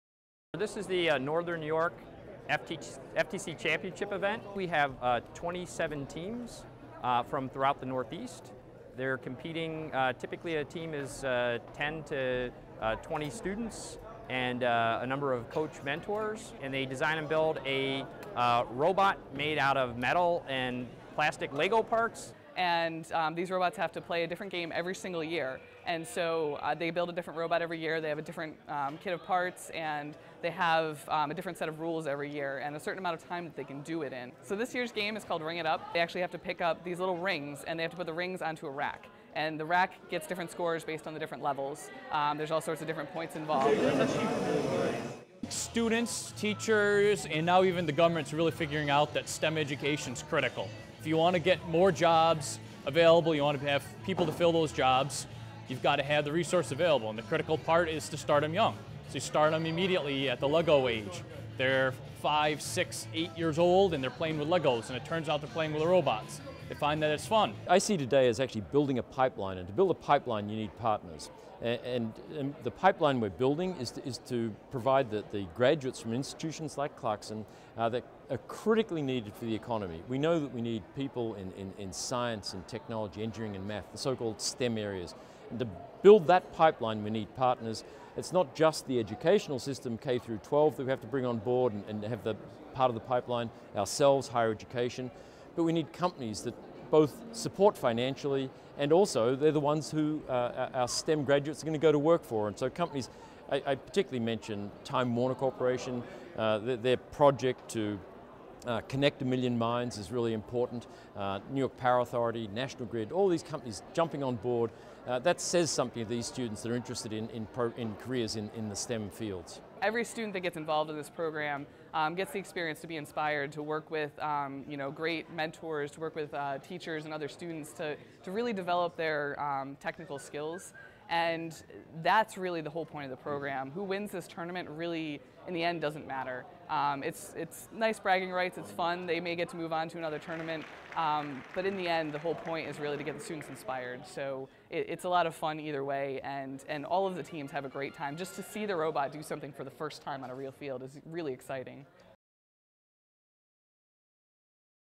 Northern New York FIRST Robotics Championships at Clarkson University
What do you get when you add 400 students and teachers and several dozen robots? Clarkson University did the math for you and the answer is success.

The sixth annual FIRST Robotics Championship Tournament is more than the average robotics competition. It is building a pipeline that supplies the local and national economy.  This can only happen when a community works together on all levels. That means the educational community is partnering with the business community and that's what Clarkson University does best.